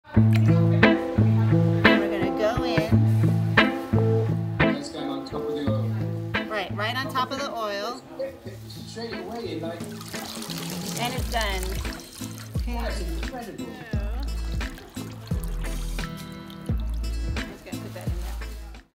How your hair can help the planet - BBC World Service
These mats are made from human hair - and they can do something amazing!